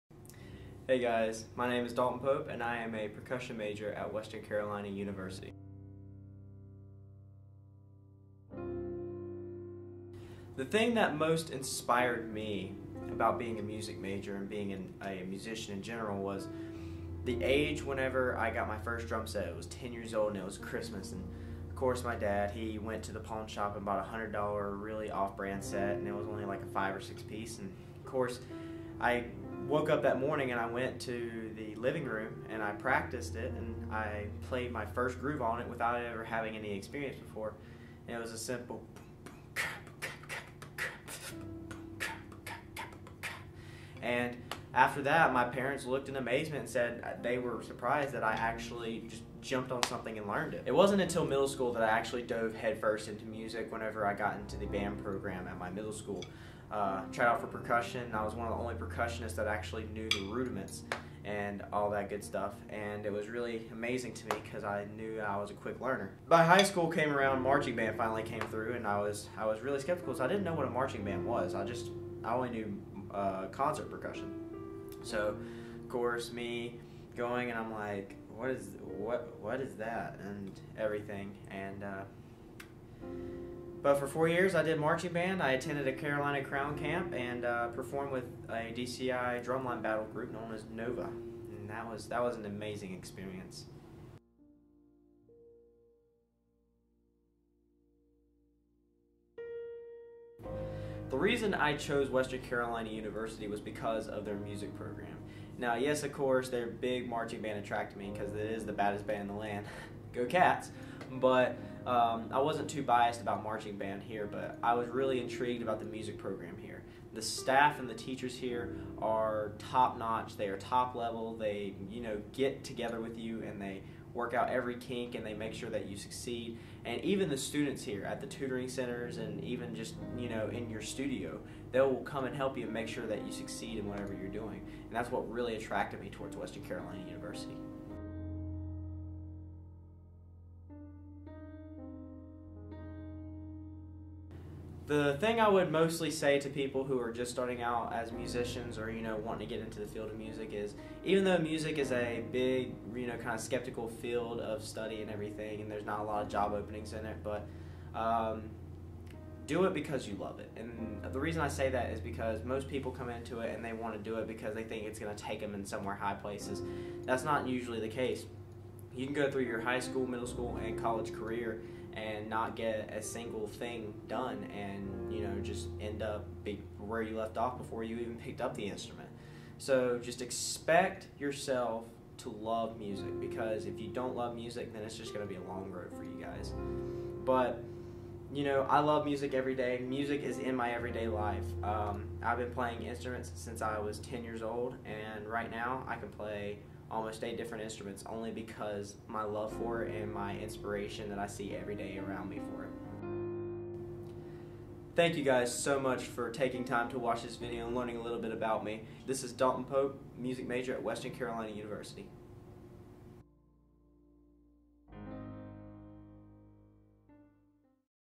DMP Studios: Why Music is my Life
I sit in the percussion studio at Western Carolina University to tell you what music means to me in my life and I give advice on upcoming music majors who plan to pursue music as a career.